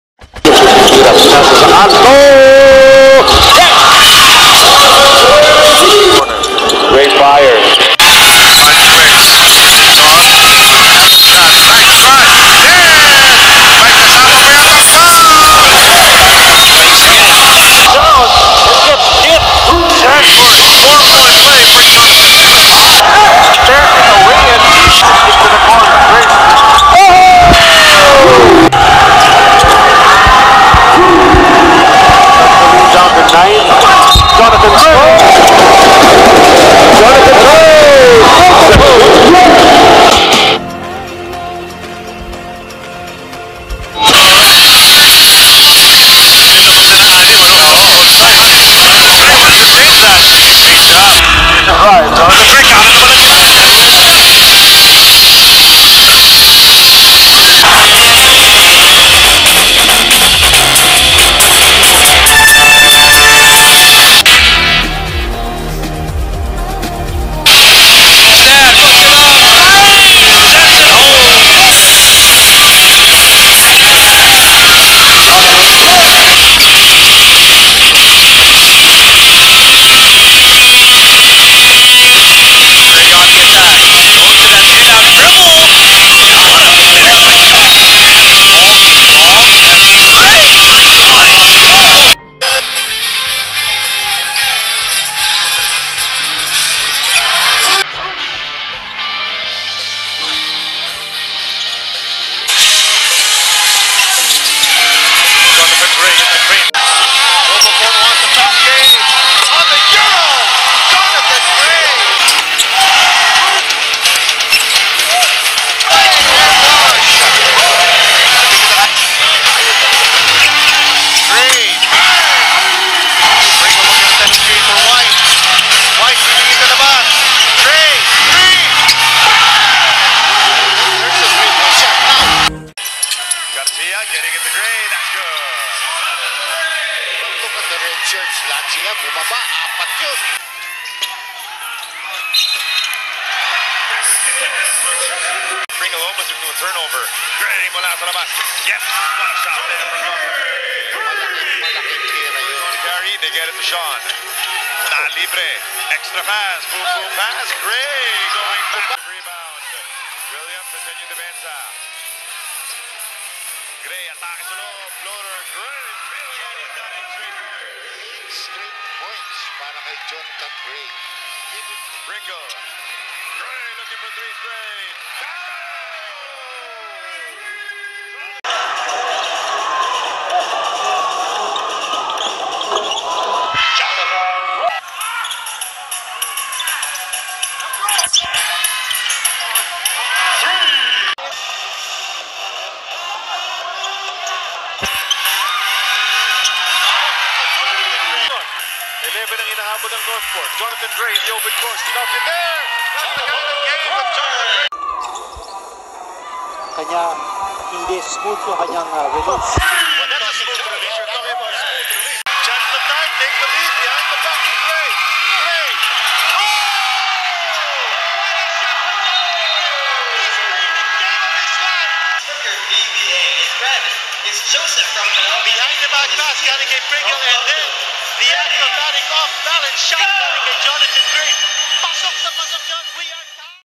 Jonathan Grey 2years in the PBA
Jonathan Grey
PBA Commissioners Cup/ Governors Cup / All Filipino Cup
Meralco Bolts #14
Globalport Batang Pier #14
Northport Batang Pier #14
Gilas Cadet